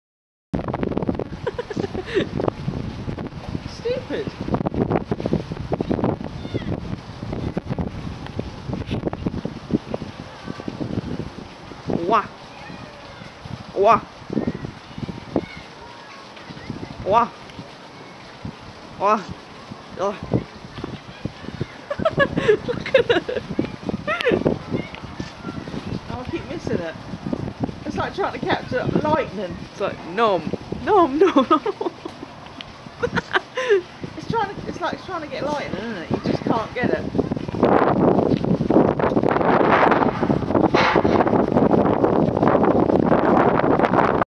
Ostrich at Colchester zoo
Ostrich acting weird, funny though!